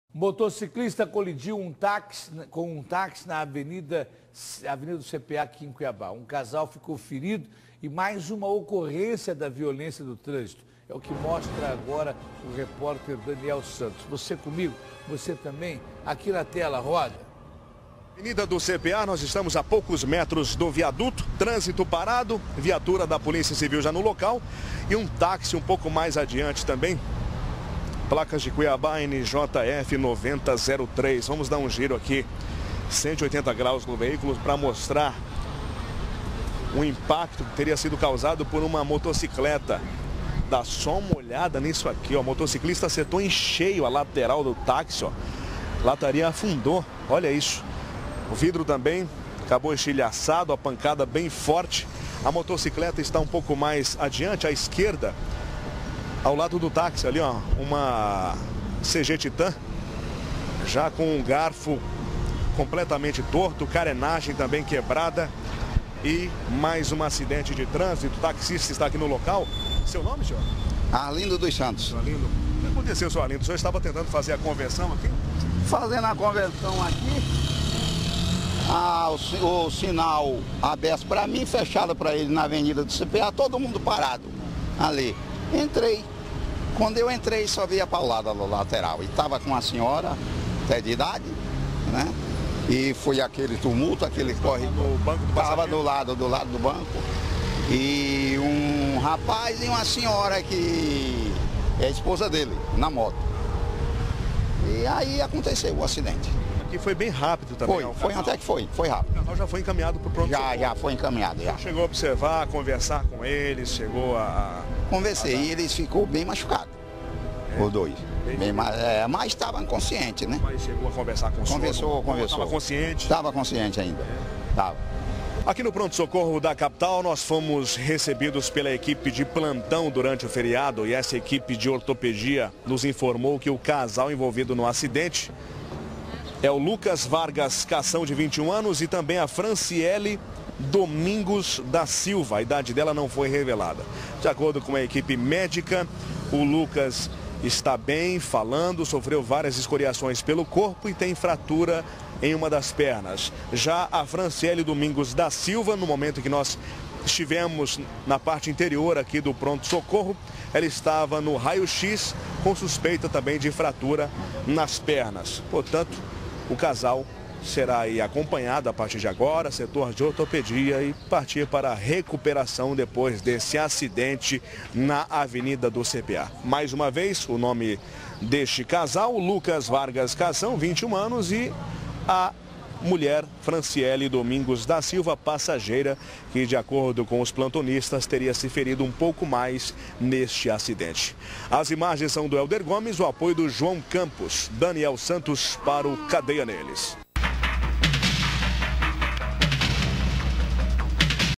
Motociclista colidi em táxi
Um casal ficou ferido e foi prestado o socorro.